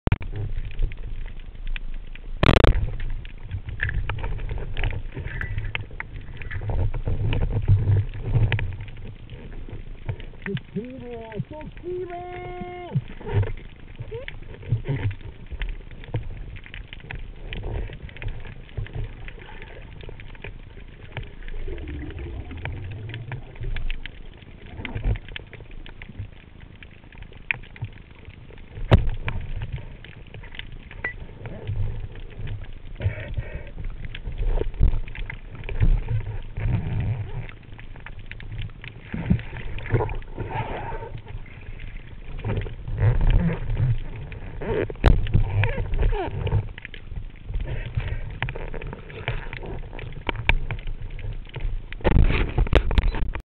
PESCA SUB , che culooo
niente da dire , faccio un aspetto , vengo su e spengo la video , poi all improvviso .